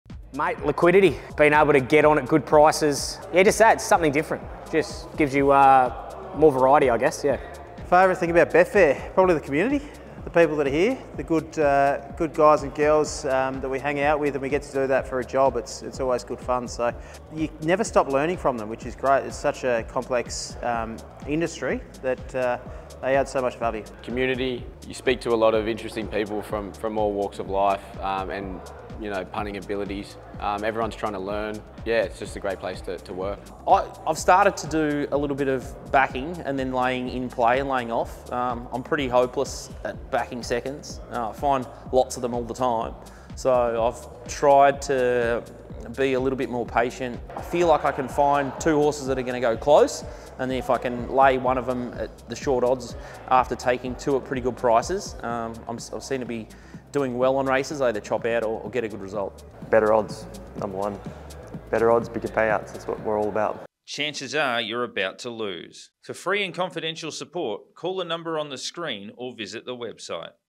Why is Betfair On Your Side?
Hear from the pros why Betfair is on the side of the punter... 🔛